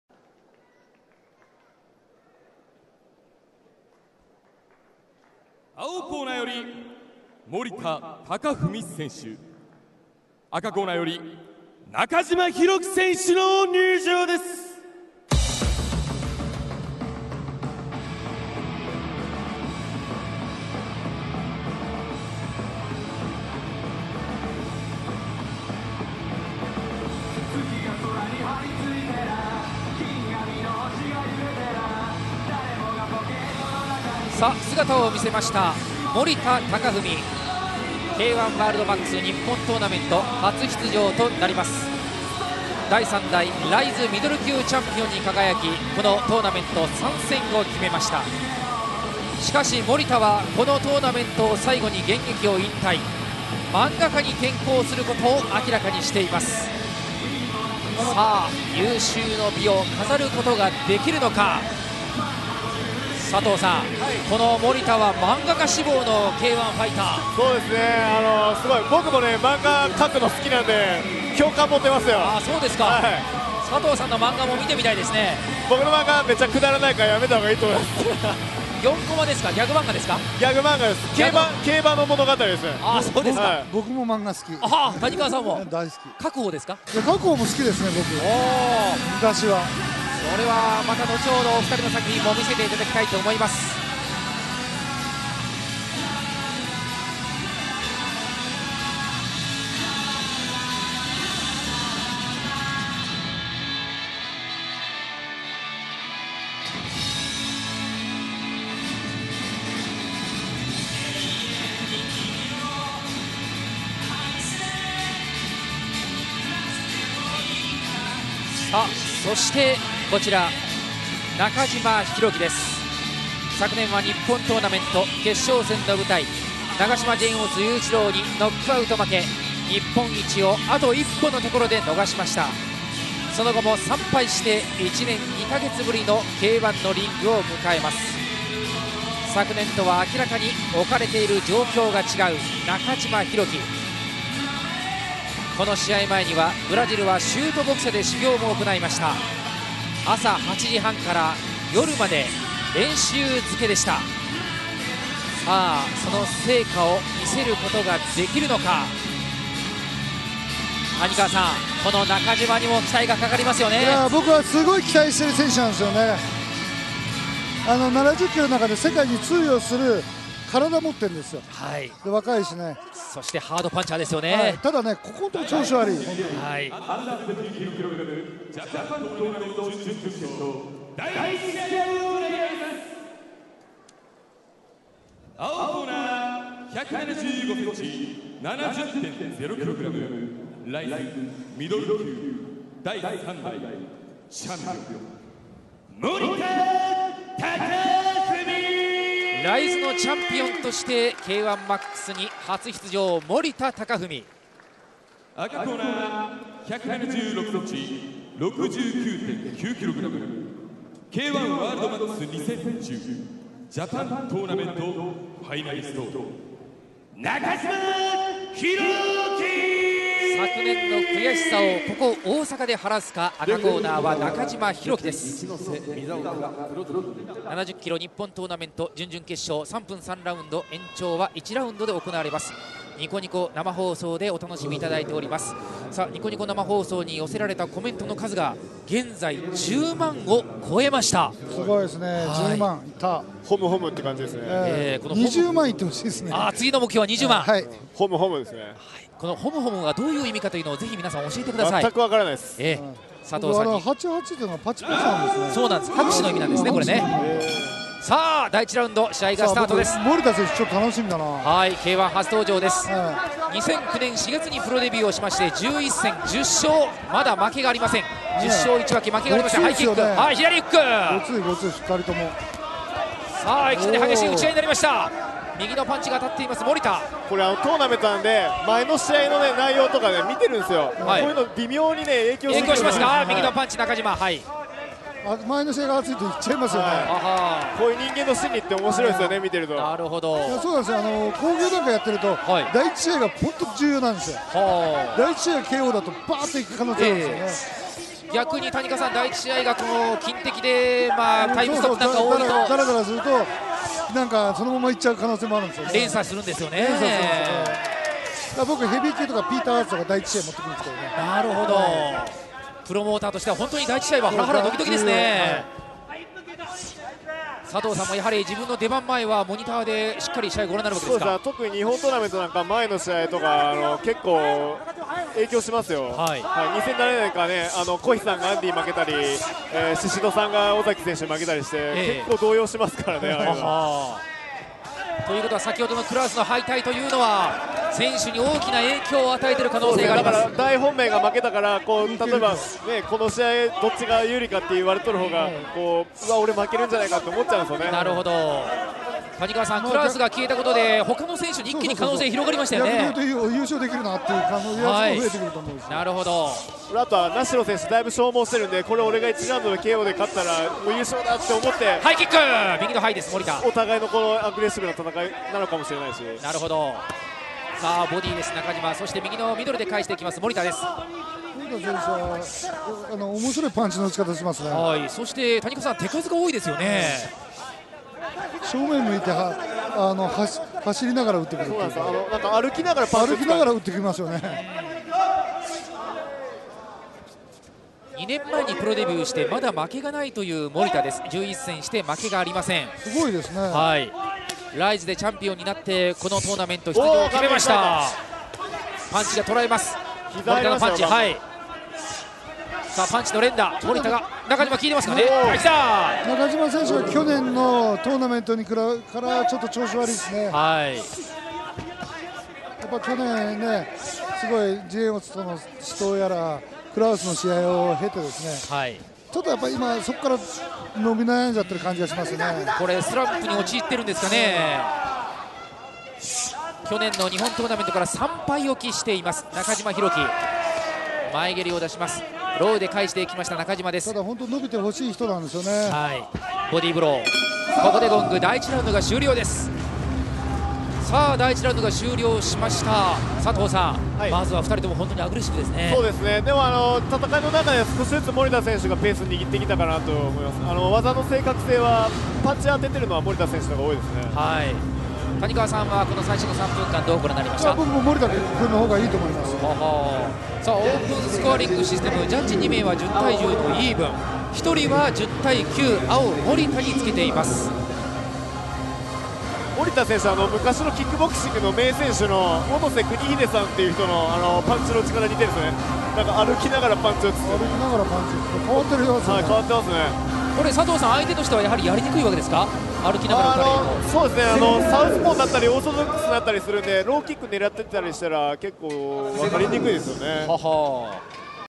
K-1 World MAX 2011 -70kg JAPAN GP QF#2 Hiroki Nakajima vs Takafumi Morita Part 1
K-1 World MAX 2011 -70kg JAPAN GP Quarter Final #2 Hiroki Nakajima(中島弘貴) vs Takafumi Morita(森田崇文) Part 1
K-1 WORLD MAX 2011 -70kg JAPAN GP FINAL
September 25th 2011
Osaka Prefectural Gymnasium
Osaka, Japan